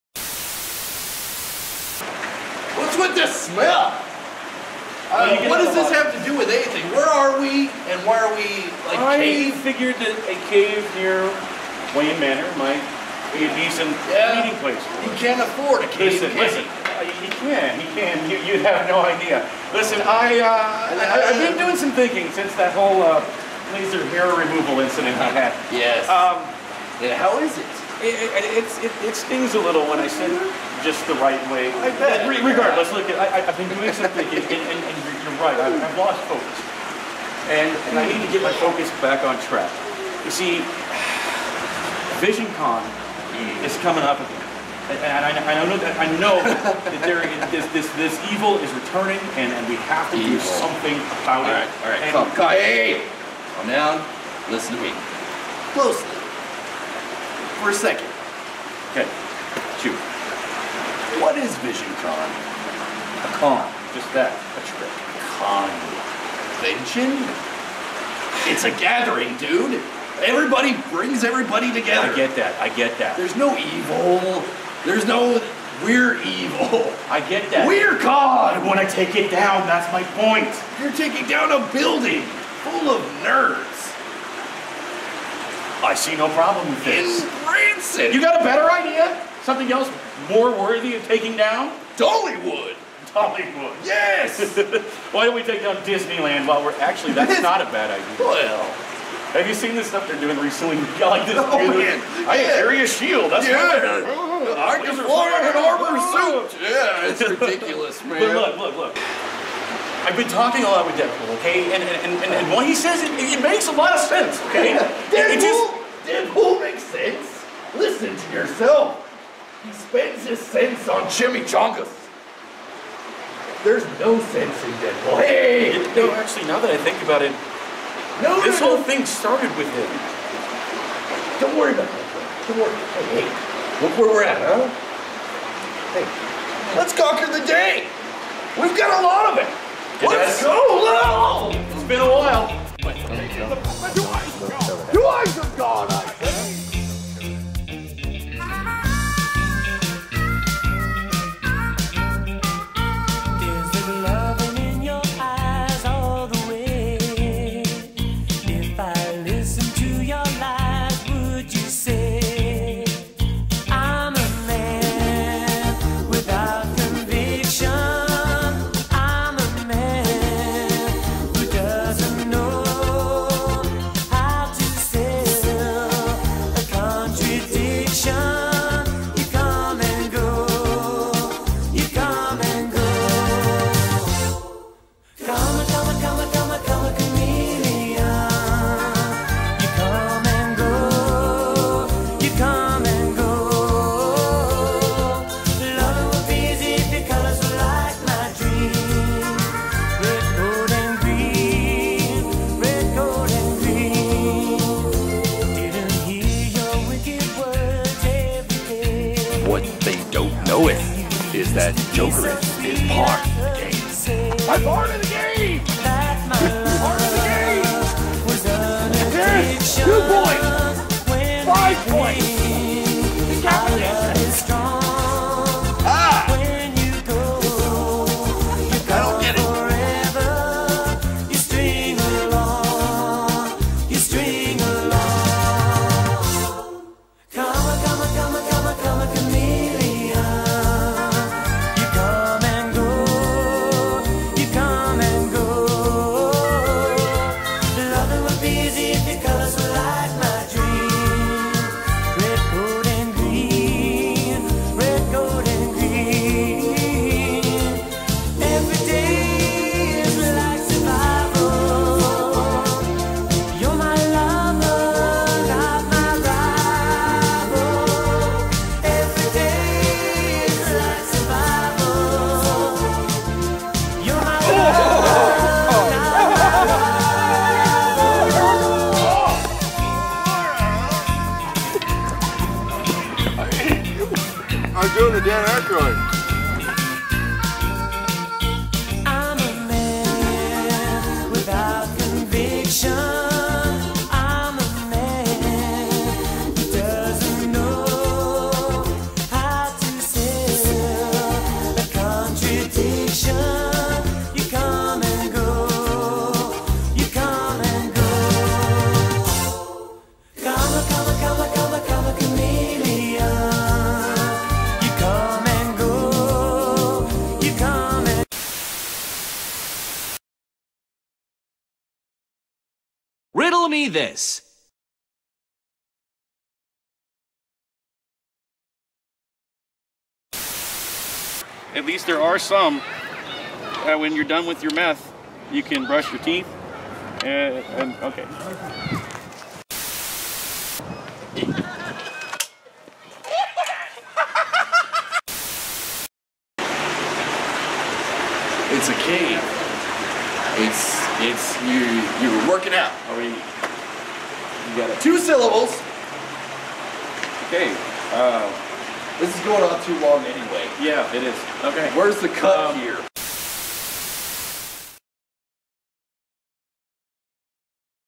The Riddler & Joker's Day at the Park 2
As Visioncon approaches, The Riddler seeks help from The Joker one last time.